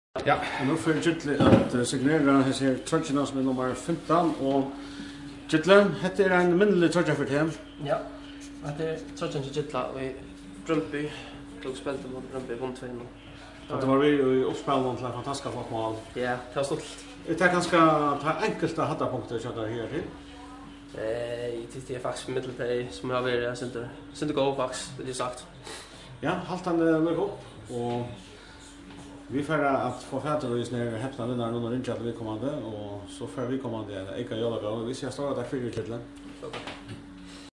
Gilli R. Sørensen signerar originala AaB troyggju (nr. 15)
Gilli R. Sørensen signerar originala AaB troyggju (nr. 15) uttanfyri studio hjá Rás2. Ein heppin lurtari á Strondum vann troyggjuna.